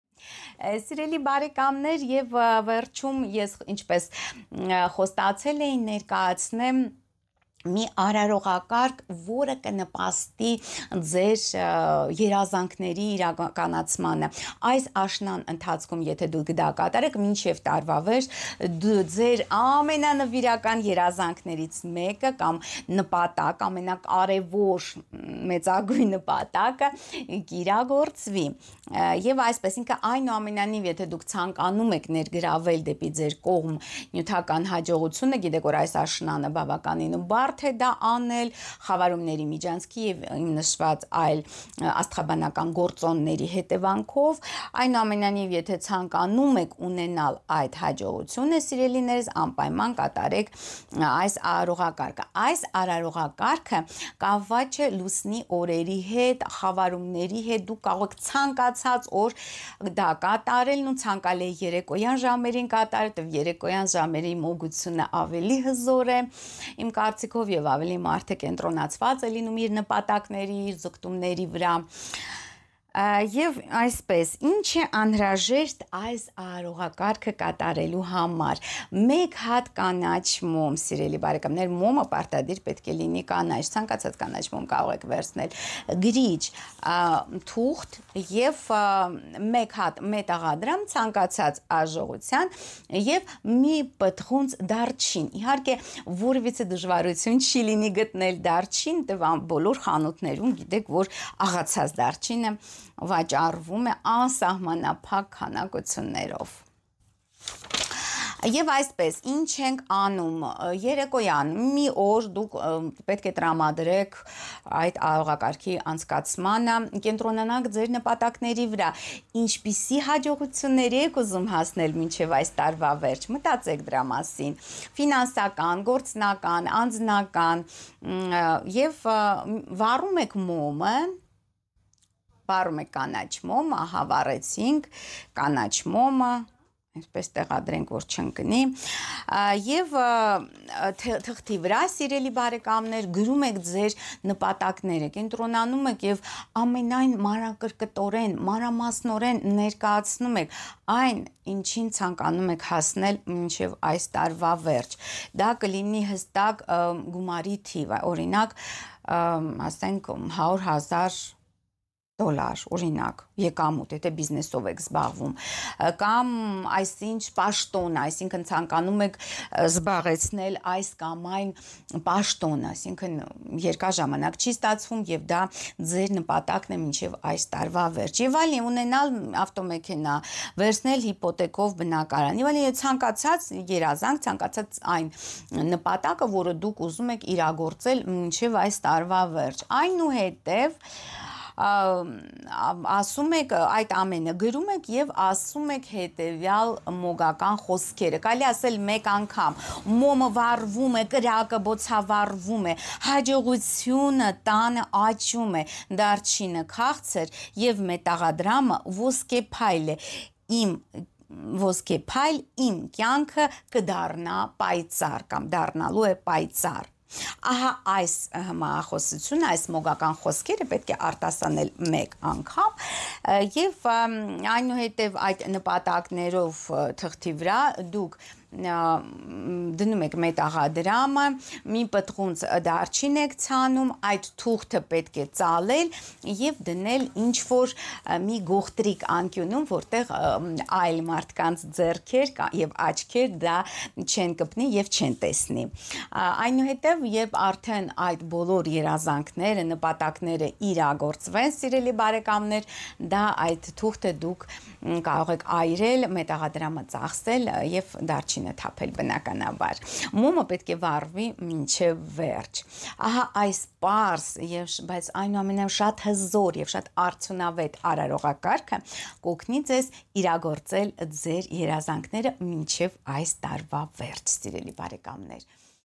Ձեր ամենանվիրական երազանքի իրագործում՝ այս աշնան ամենահզոր արարողակարգը
Իրագործք ձեր ամենանվիրական երազանքը մինչև 2024 թվականի վերջը, այս պարզ, բայց շատ արդյունավետ արարողության օգնությամբ։
Ձեզ պետք կգա մեկ հատ կանաչ մոմ, թղթի կտոր, գրիչ, մի պտղունց դարչին և մեկ հատ մետաղադրամ։
Անհատական խորհրդատվության համար զանգահարեք՝